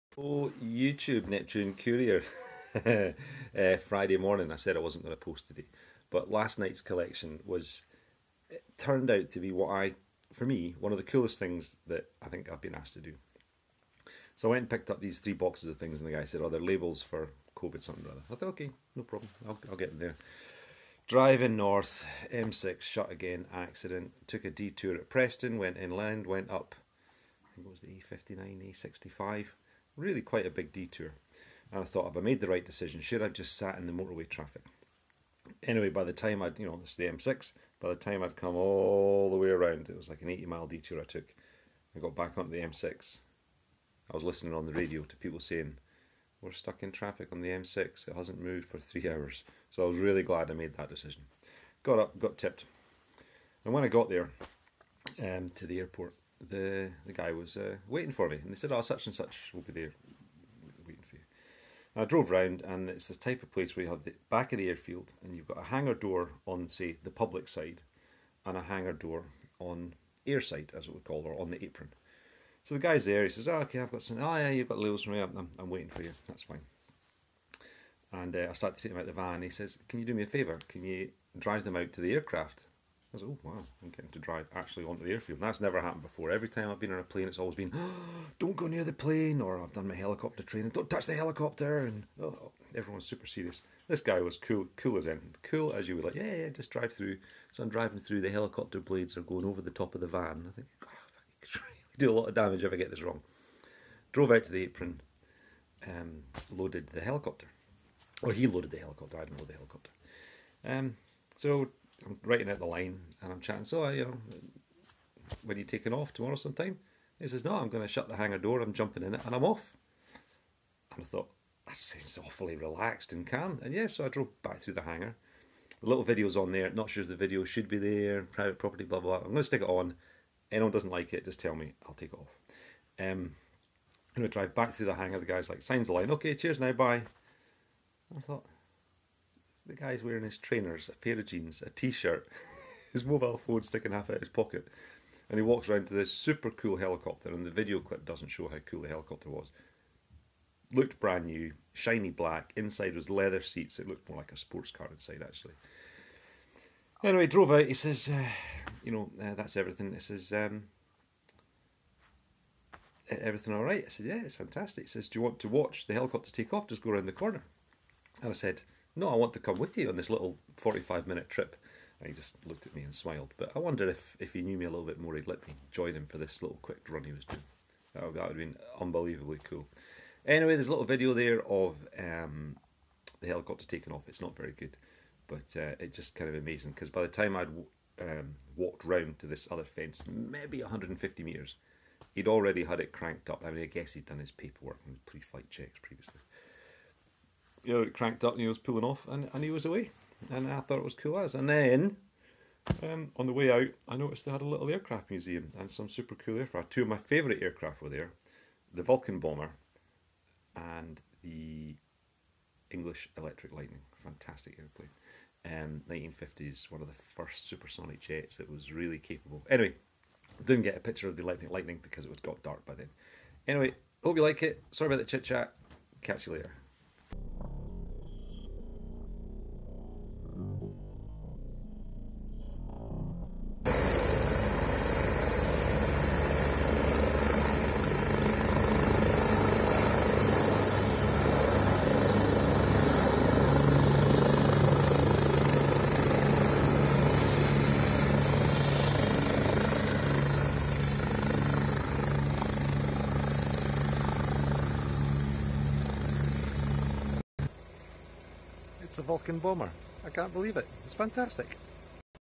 Neptune Courier, delivery to a helicopter with a Vulcan Bommer vet on display
Dropping off to a heli base and they had a Vulcan Bommer on display just outside.